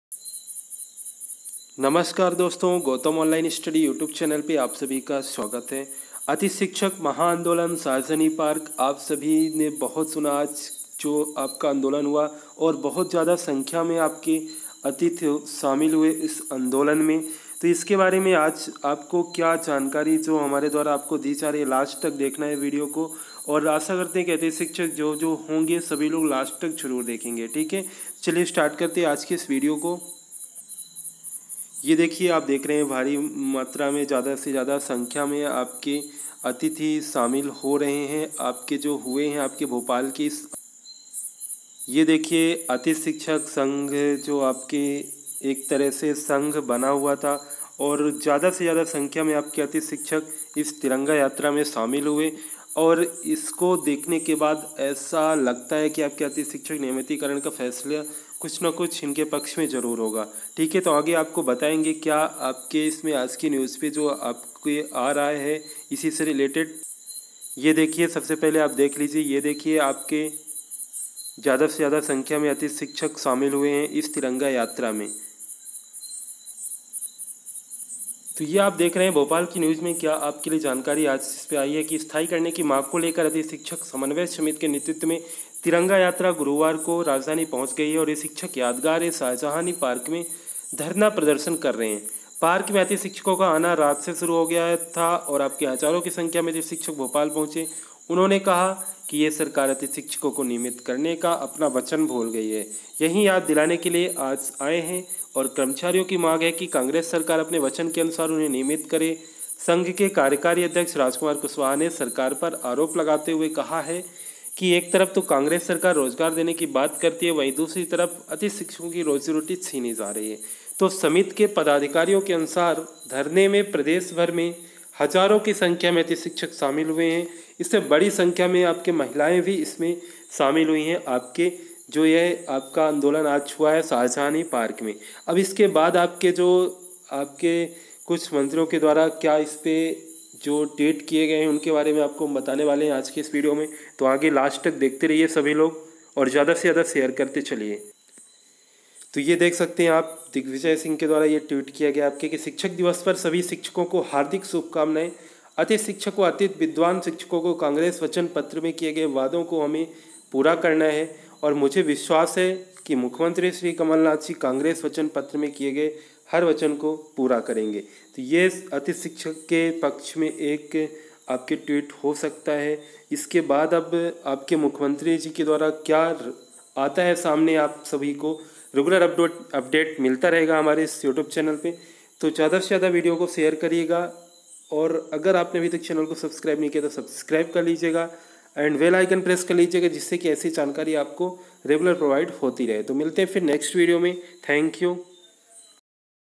अतिथि शिक्षक महाआन्दोलन नियमतिकरण 5 सितंबर इसके बाद क्या फैसला आ सकता है - देखिये
Welcome Channel
Gautam Online Study

वर्ग_2 रिजल्ट